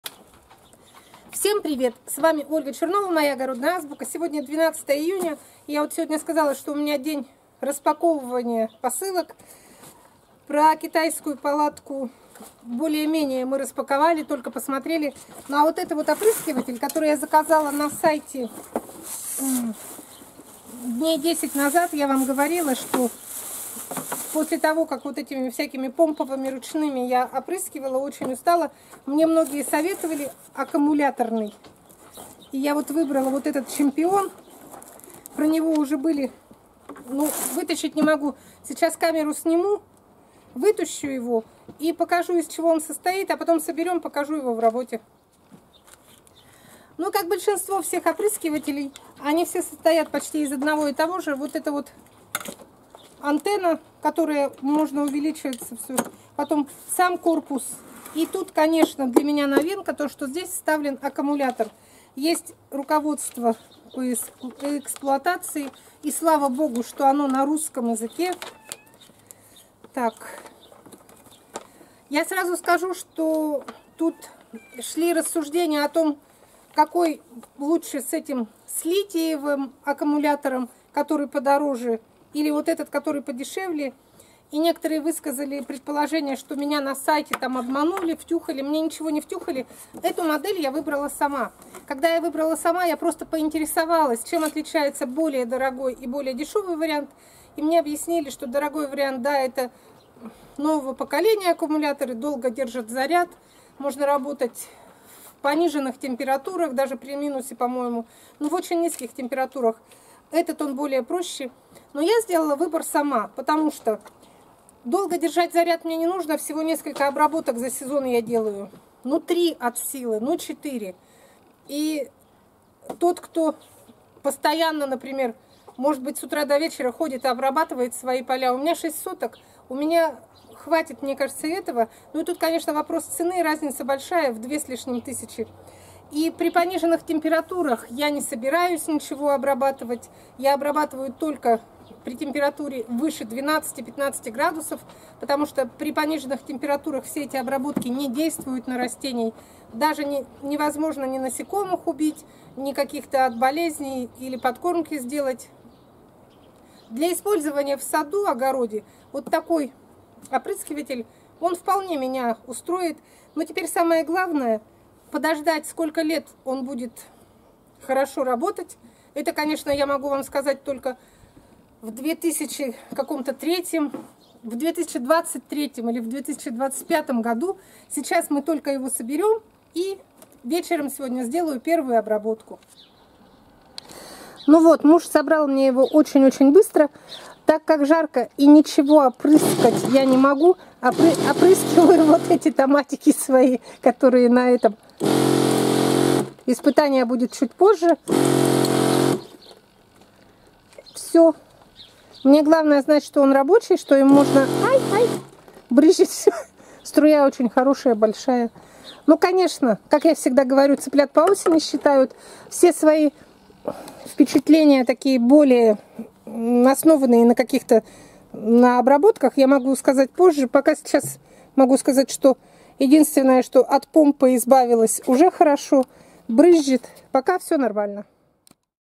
КУПИЛА АККУМУЛЯТОРНЫЙ ОПРЫСКИВАТЕЛЬ "ПАТРИОТ". Ольга Чернова.
Наконец-то я купила себе аккумуляторный опрыскиватель. Как я его выбирала смотрите по ссылке Как я выбирала опрыскиватель. 31 мая.